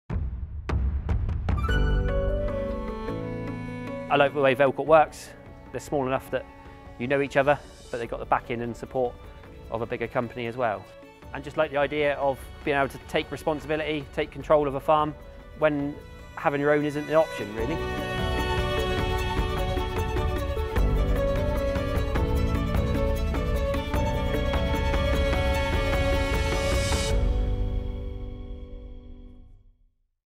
Ross Edwards, Dairy Farm Manager, on Velcourt's MTS
Velcourt's Management Training Scheme is considered as an industry leading pathway for aspiring farm managers of the future.